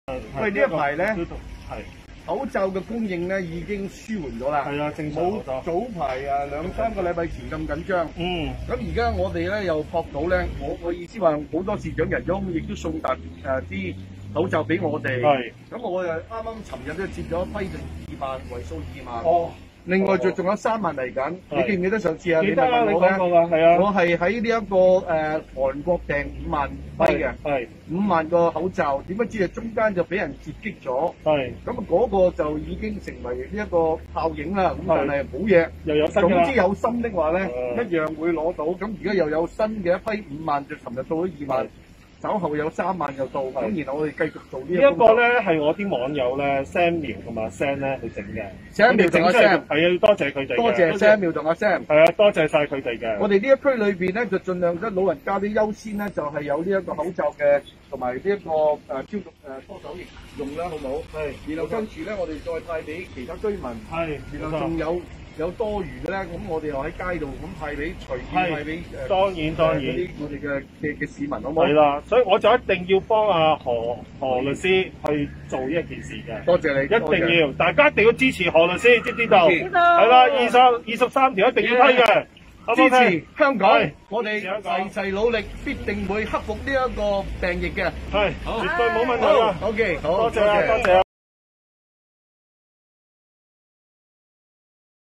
建制戰狼抗疫送暖 何君堯街頭派口罩 感謝善心仁德！
新冠肺炎肆虐，雖然近日口罩供應不似近期緊張，但仍有很多弱勢社群一罩難求，有善心人士，為他們到處搜集口罩，希望在疫情下幫助他們得到最基本的保護。建制派戰狼何君堯亦不甘後人，雖然搜購行動障礙重重，依然無阻他的善心，日前他就搜羅到一批口罩，在街上派發給巿民，並且不忘推動23條，一心多用，戰狼好嘢！

原片：何君堯 (Junius Ho Kwan Yiu) Facebook